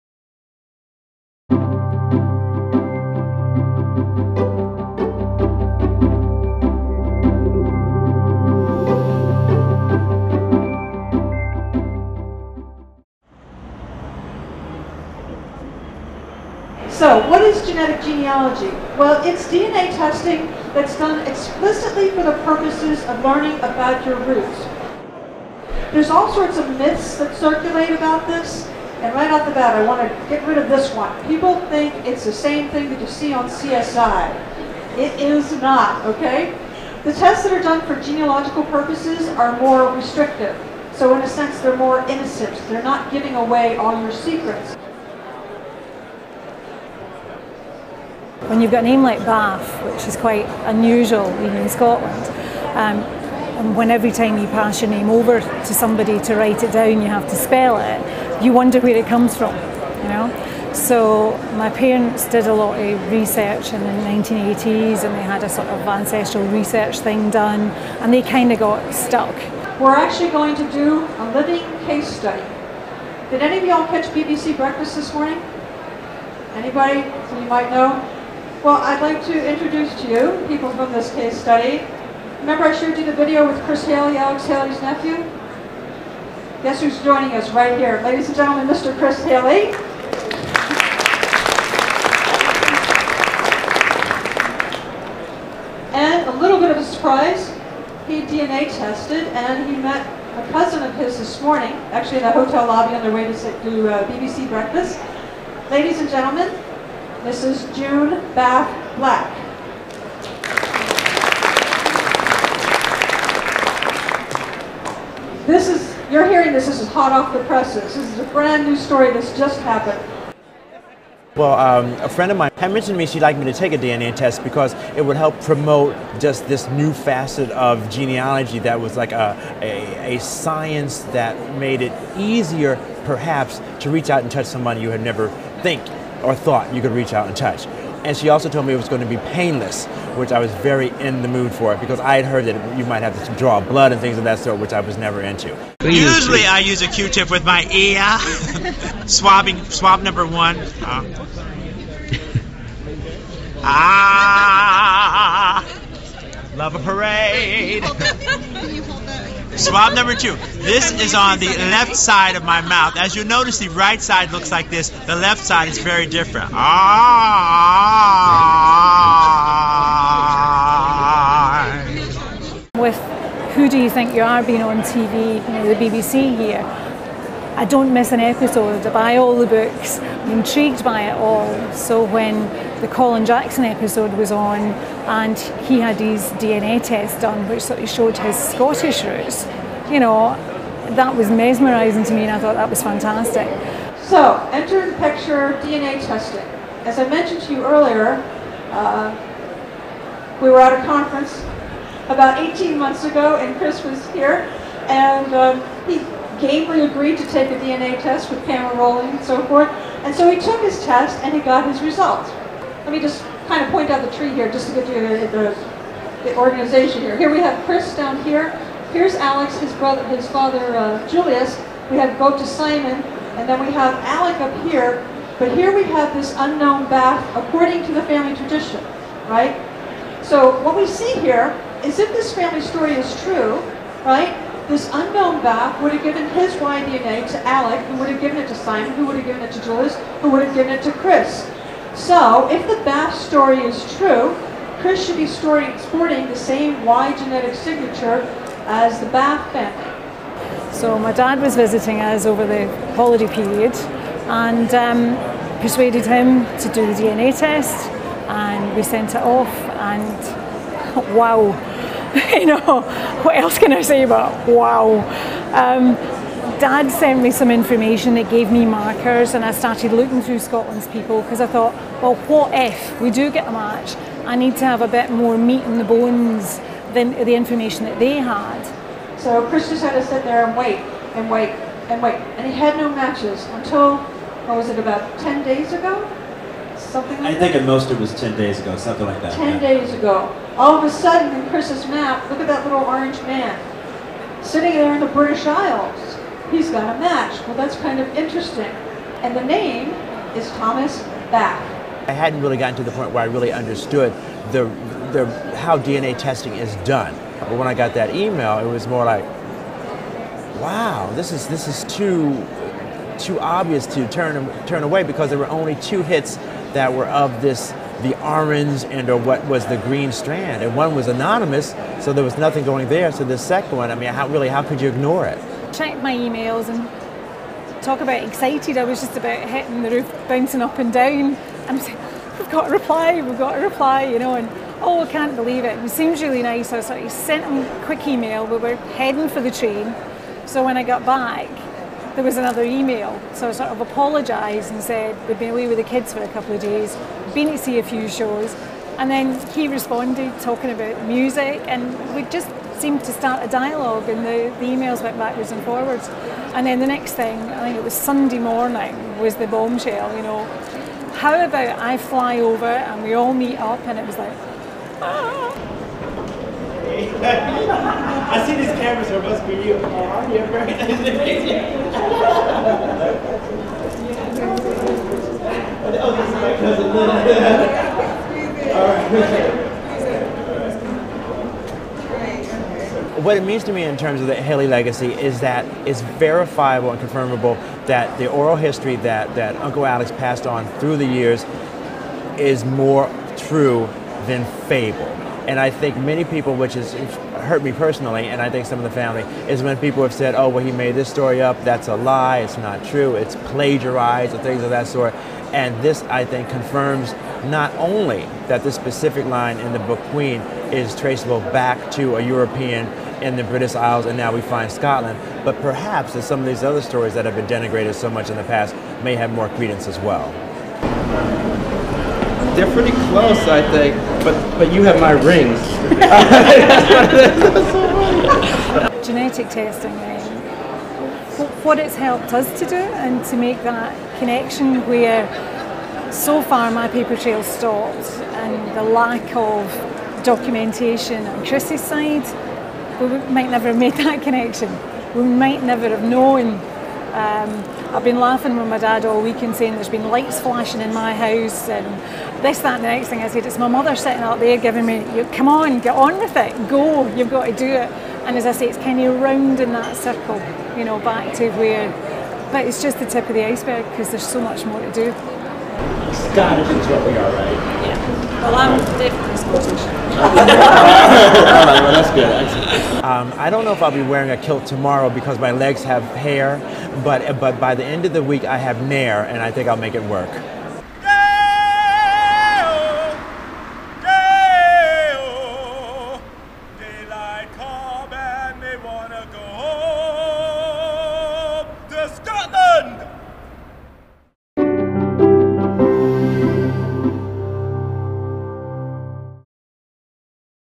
Genetic Family Reunion: DNA Reveals Alex Haley's Scottish Roots
Alex Haley of "Roots" fame gets much of the credit for igniting the global genealogy craze, but even he probably couldn't have imagined what DNA would reveal about the Haley branch of his family. Watch his nephew, Chris Haley, meet his Scottish cousin, June Baff Black, while attending the Who Do You Think You Are LIVE! event in London. Along the way, genealogist Megan Smolenyak explains how Y-DNA testing can be used to solve family histories mysteries (perhaps you have one that could be solved this way), and you'll probably be left wondering whether Chris's powerful lungs (singing over London traffic) are genetic, too!